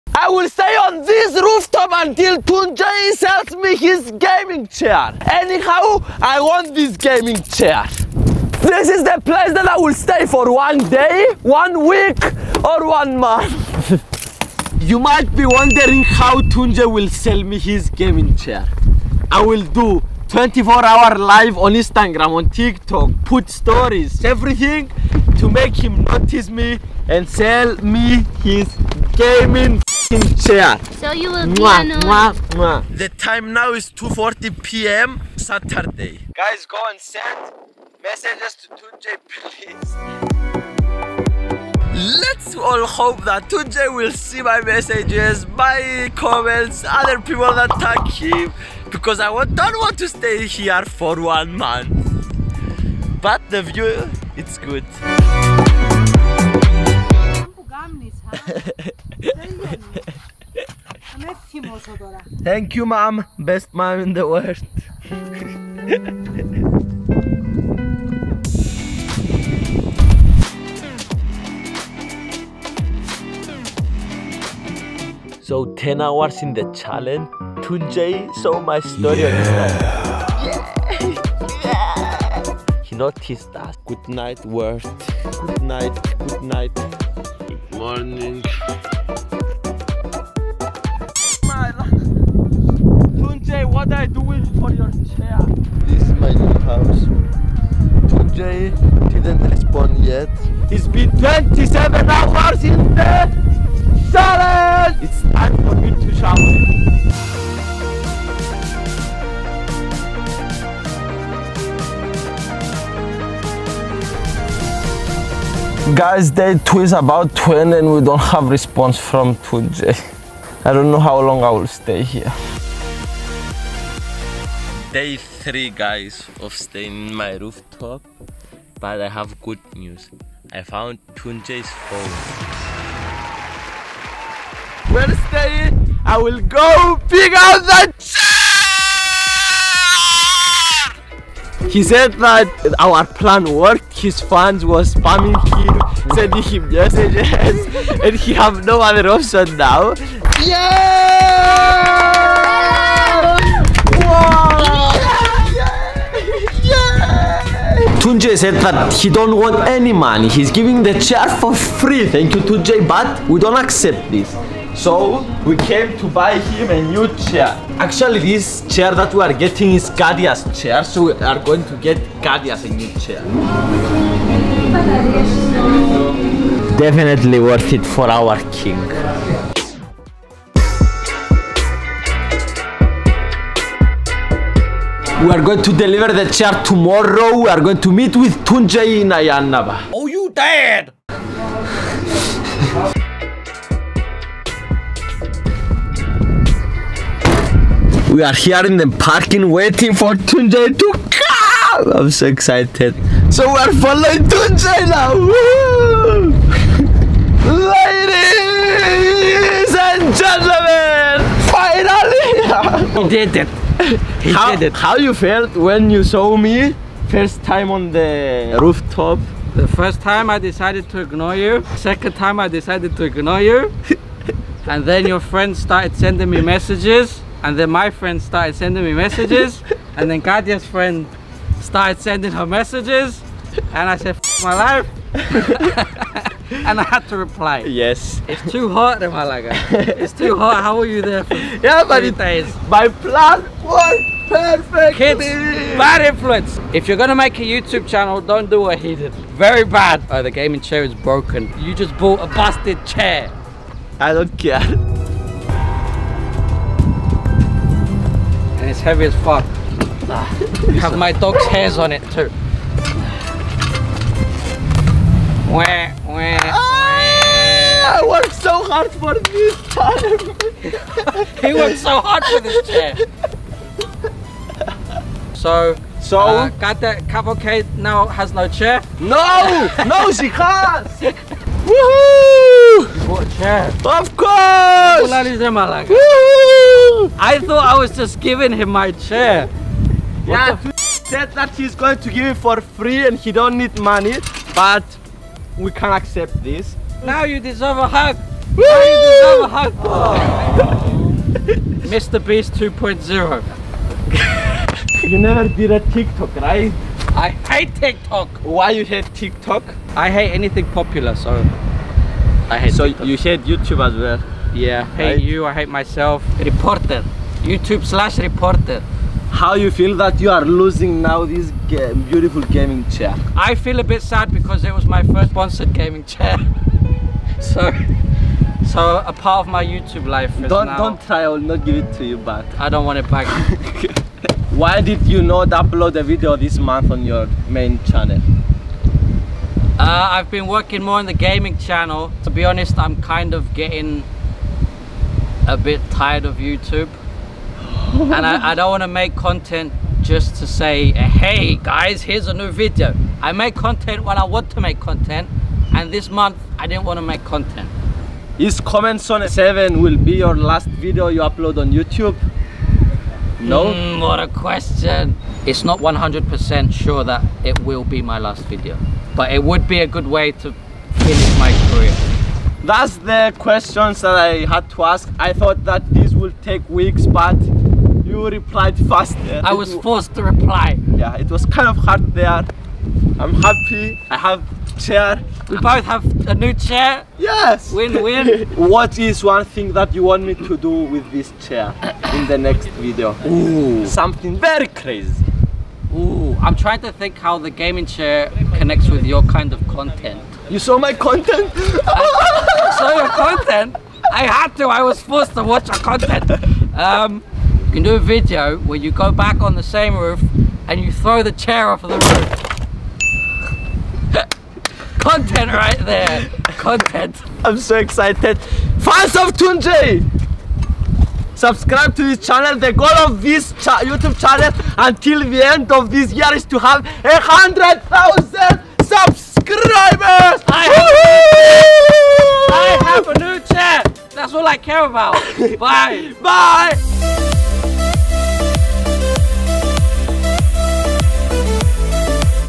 I VISITED 2J's HOUSE AND GOT HIS CHAIR!
2J will hate me after he see this video...
BeeCool is all about making being weird, crazy and different cool!

I was 50 hours on my rooftop to get 2j's attention. I managed to go to 2j place to get his gaming chair! That chair belongs to cat von k and she was too generous to give it to me. It was win win for Me and 2j.

Edited by: Laxminath dangol